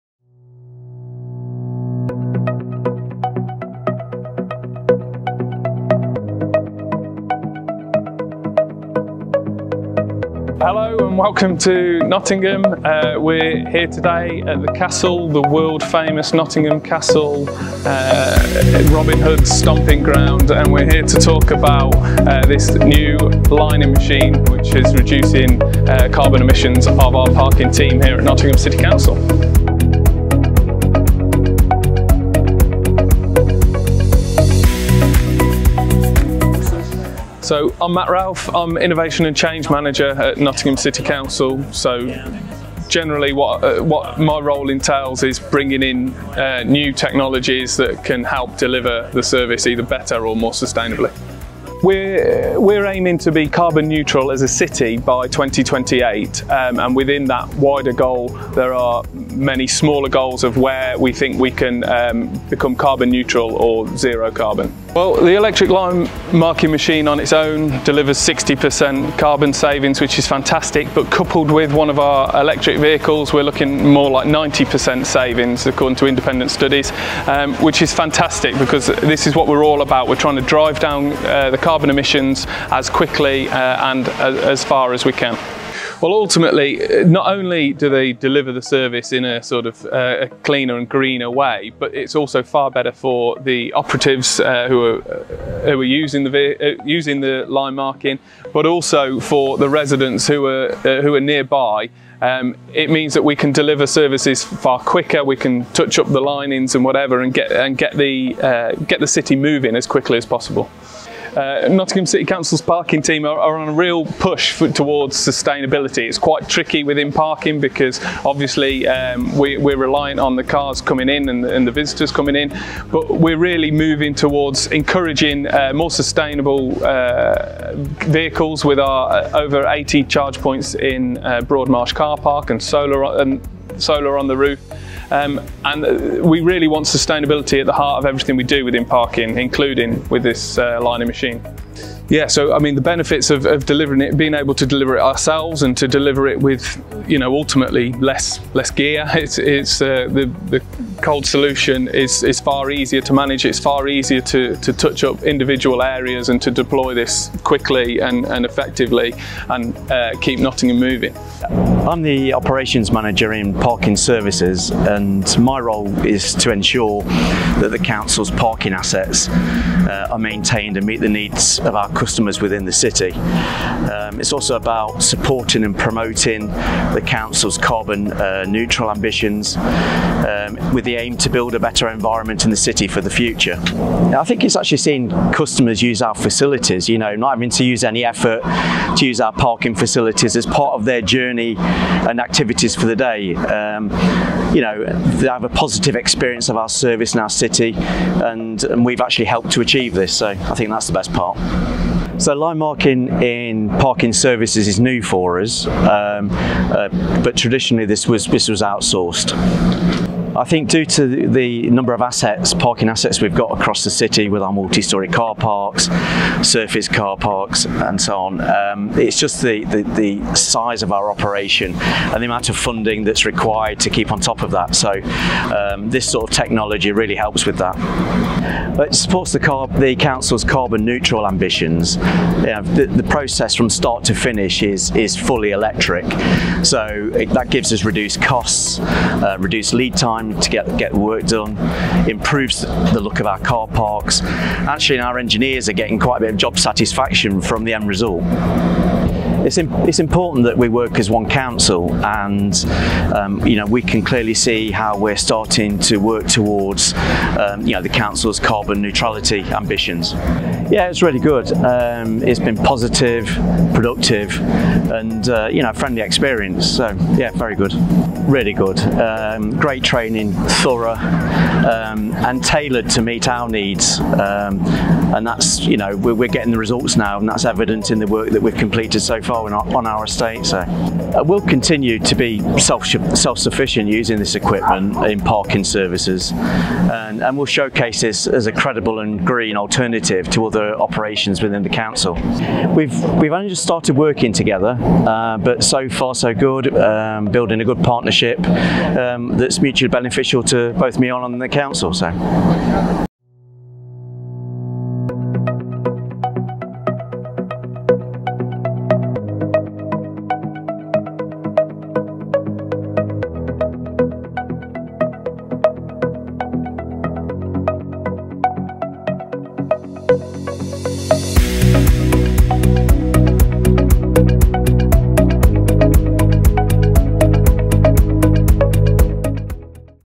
Nottingham City Council Case Study | Supporting the Council's Decarbonisation Goals
Nottingham City Council are always actively looking for ways to drive down carbon emissions and provide their public services with the most efficiency, whilst staying as green as possible.

Nottingham is on its way to becoming a carbon neutral city by 2028, and by implementing changes and meeting smaller objectives of their decarbonisation strategy, this goal is within sight.

Matt Ralfe, The City Council’s Innovation and Change Manager, has the responsibility to ensure new sustainable technologies are being implemented within the day-to-day operation of Nottingham City Council’s services.

“We’re aiming to be carbon neutral as a city, by 2028. Within that wider goal, there are many smaller goals where we think we can become neutral. We are trying to drive down carbon emissions as quickly and as far as we can.”

Matt Ralfe - Innovation and Change Manager at Nottingham City Council